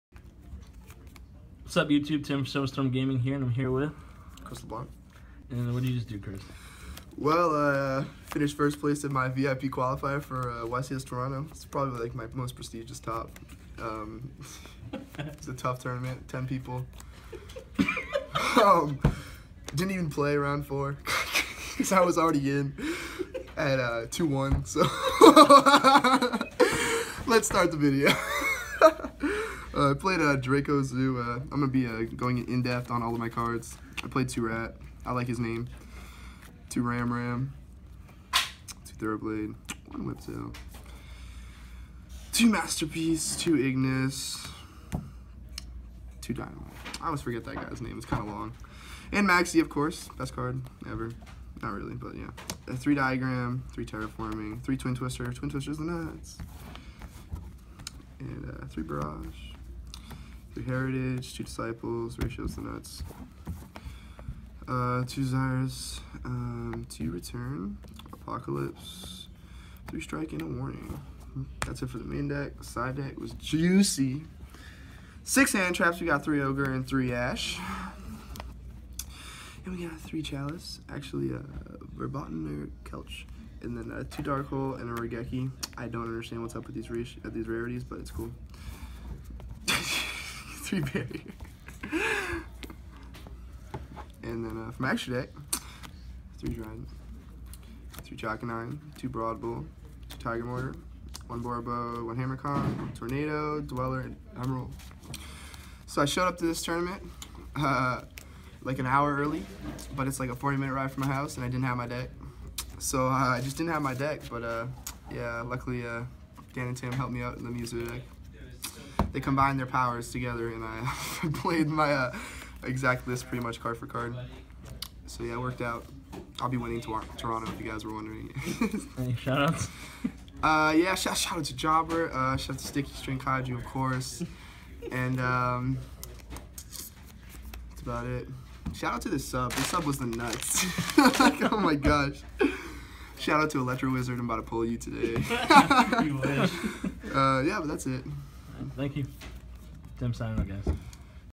Chris Leblanc 1st place Yu-Gi-Oh! VIP qualifier 2017 (Toronto)
Check out the deck Chris Leblanc used to win the VIP qualifier. Very laid back and fun profile. His build was super consistent and very powerful.